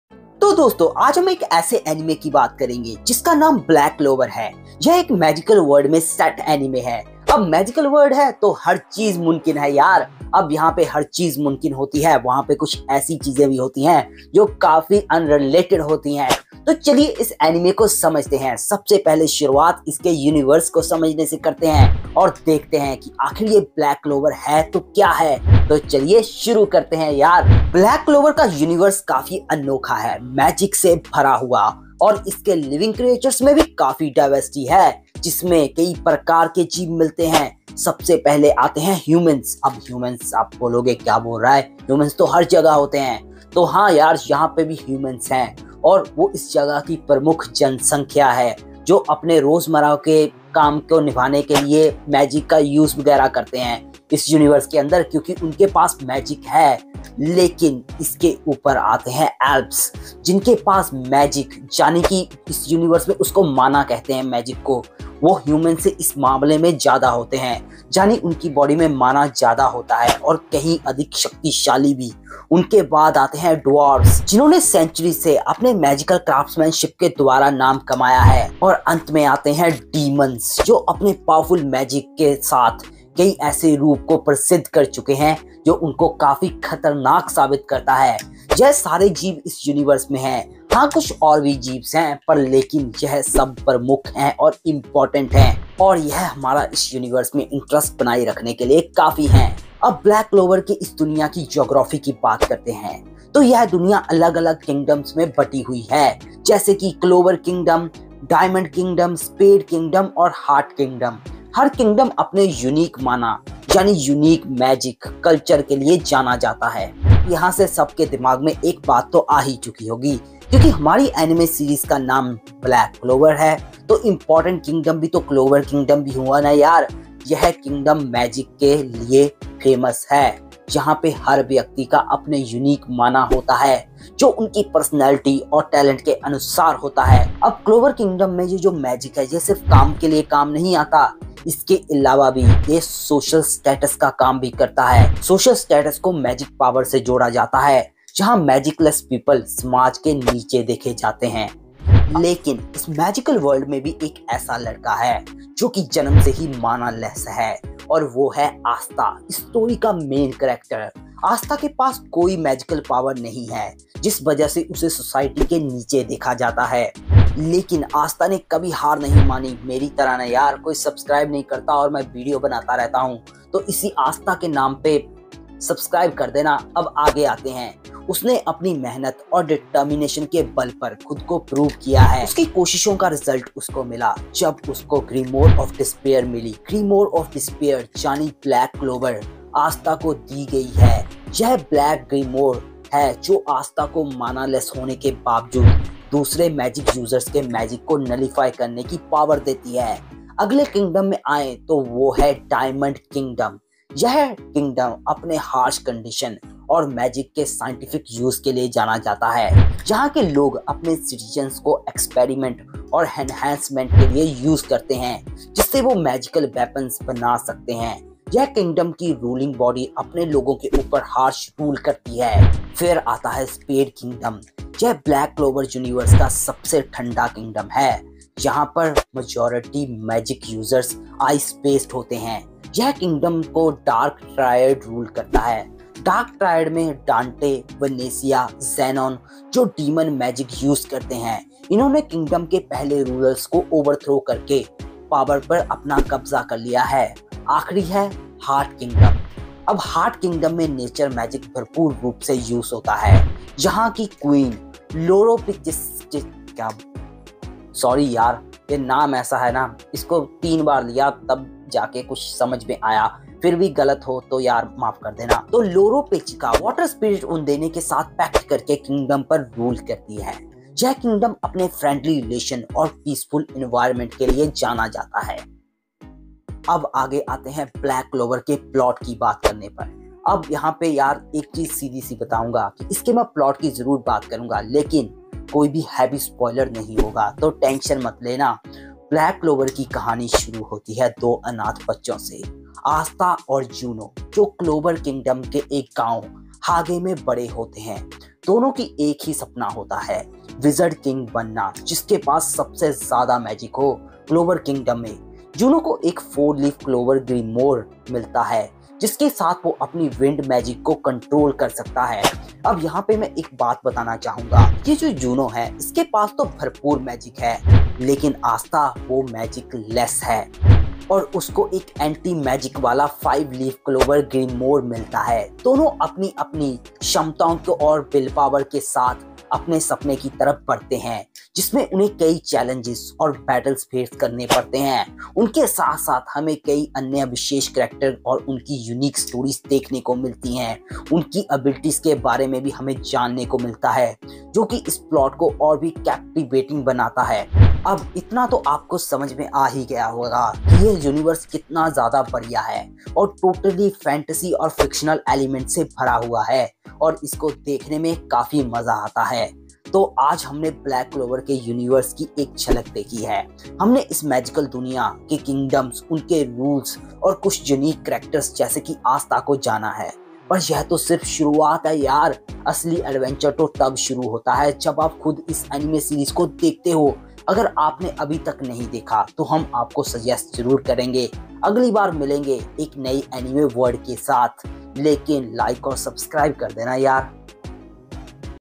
Discovering the Magical Universe of Black Clover: An Ultimate Guide in Hindi
Title: "Discovering the Magical Universe of Black Clover: An Ultimate Guide"

Description:
Dive into the mystical universe of 'Black Clover' and experience the wonders of magic in this detailed guide. We'll explore the different kingdoms, their unique magical elements, diverse creatures, and the inspiring journey of Asta, a character who defies all odds. Whether you're a seasoned anime fan or a newcomer to the world of 'Black Clover', this video is for you.